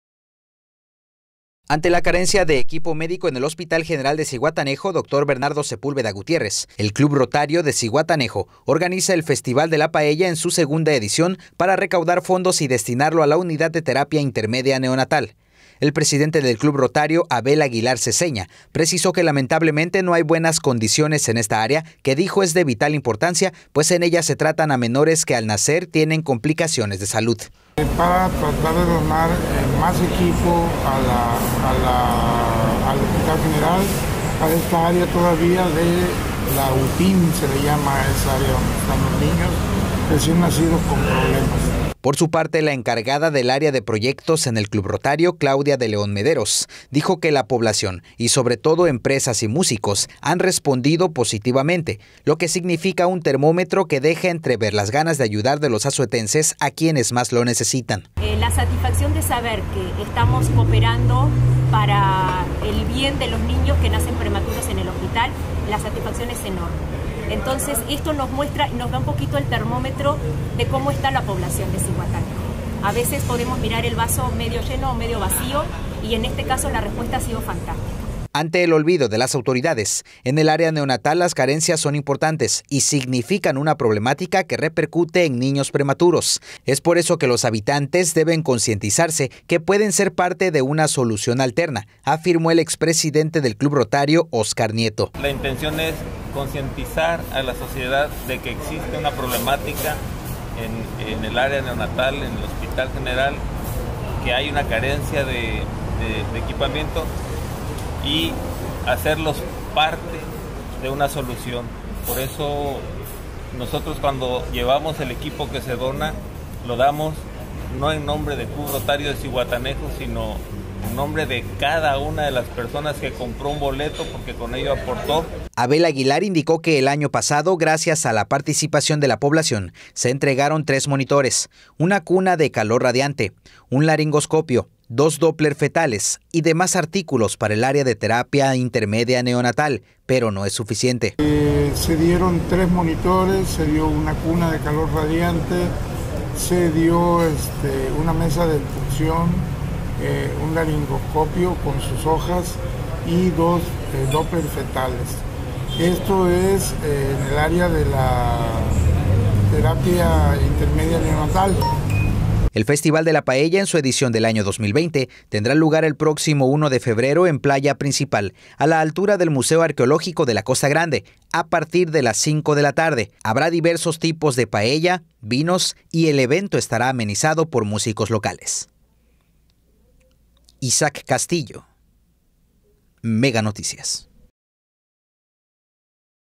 Club Rotario busca equipar “UTIN” del Hospital General
Ante la carencia de equipo médico en el Hospital General de Zihuatanejo "Doctor Bernardo Sepúlveda Gutiérrez", el Club Rotario de Zihuatanejo organiza el Festival de la Paella en su segunda edición para recaudar fondos y destinarlo a la Unidad de Terapia Intermedia Neonatal.